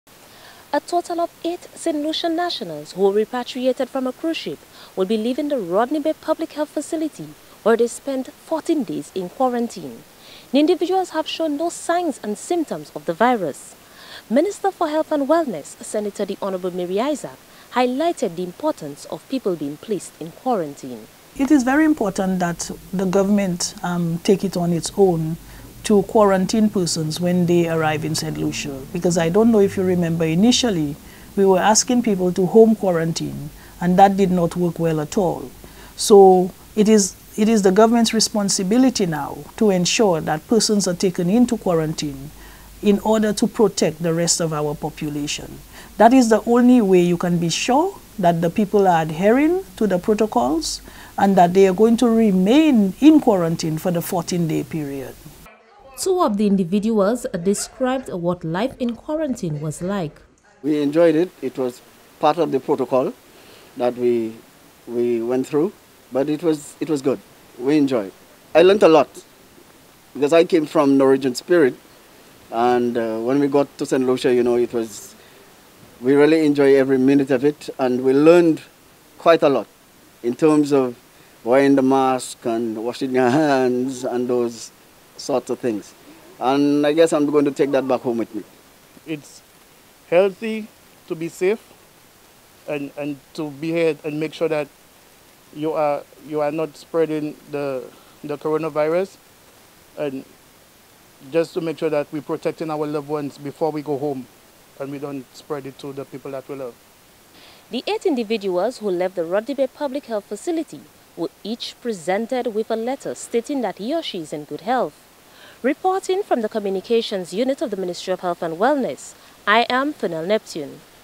Eight nationals released from quarantine
A total of eight Saint Lucian nationals repatriated from a cruise ship were released from the Rodney Bay Public Health Facility after spending 14 days in quarantine. The individuals showed no signs and symptoms of COVID-19.